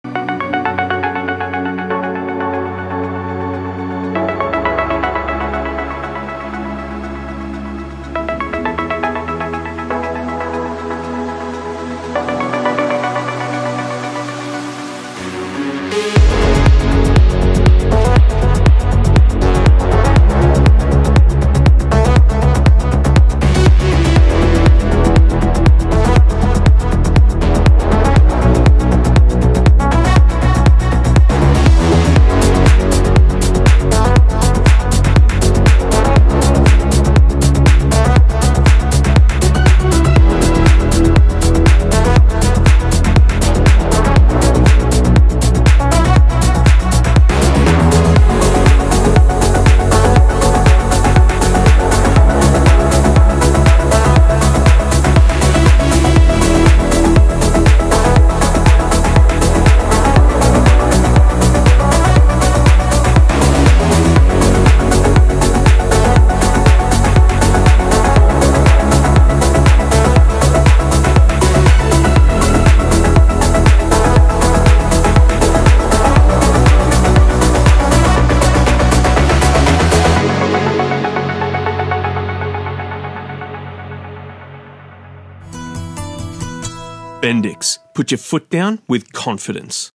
Cars of Bendix - Unmarked VOL.II
In this month's edition of Cars of Bendix we checked out Sydney's newest premium event, Unmarked VOL.II! Packed to the brim with the absolute cleanest builds that Sydney has to offer, Unmarked did not disappoint! Check out the full video and see for yourself!

For more information on Bendix Performance Brake Products head over